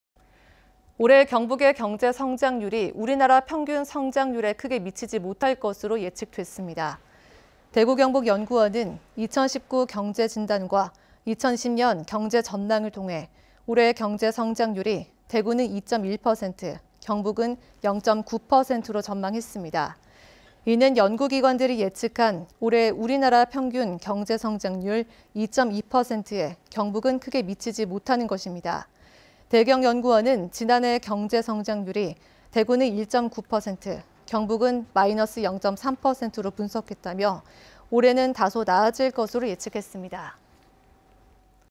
올해 경제성장률 경북 0.9%[포항MBC뉴스]
올해 경북의 경제성장률이  우리나라
평균 성장률에 크게 미치지 못할 것으로
예측됐습니다
           ◀ＥＮＤ▶

대구경북연구원은 '2019 경제진단과
2020년 경제 전망'을 통해
올해 경제성장률이 대구는 2.1%,
경북은 0.9%로 전망했습니다

이는 연구기관들이 예측한 올해 우리나라
평균 경제성장률 2.2%에,
경북은 크게 미치지 못하는 것입니다

대경연구원은 지난해 경제성장률이
대구는 1.9%,경북은 마이너스 0.3%로
분석했다며,올해는 다소 나아질 것으로
예측했습니다